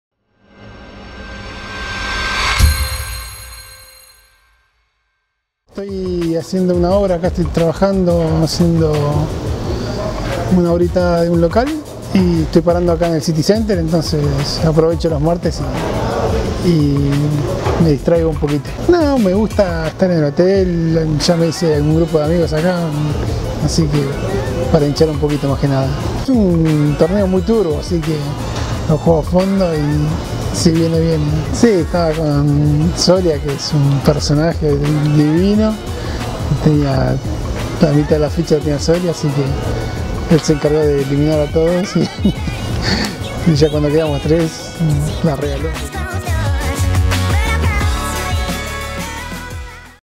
Tuesday Poker City: Entrevista a Fernando Di Francesco subcampeón
Entrevista a Fernando Di Francesco subcampeón, en la fecha #8 de la 10º edición del Tuesday Poker City en el City Center Rosario de Argentina.